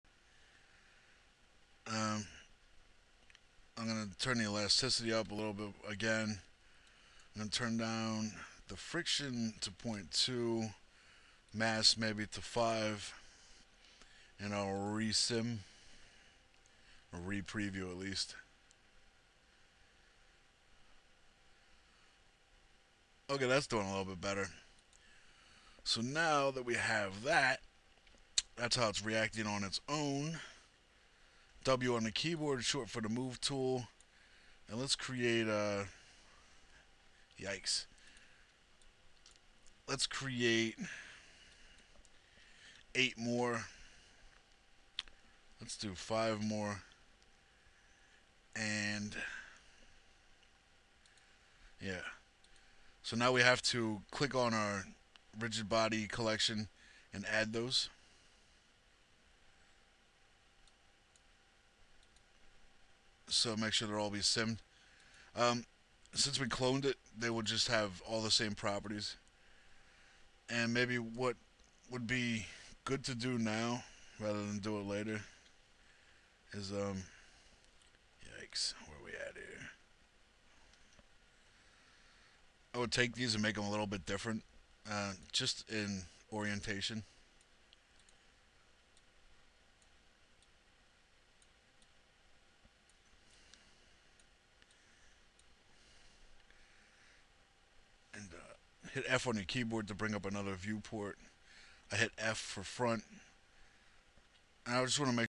Falling Shells (Tutorial) 6/13
We will work in 3ds max and photoshop to make 9mm shells that we will drop using reactor. We will then use particle flow to add particles and space warps to simulate smoke. Plus we will cover how to render in passes so we can compile all of the elements separately. Lastly we will be using both scanline and mental ray renderers. . .

_gawn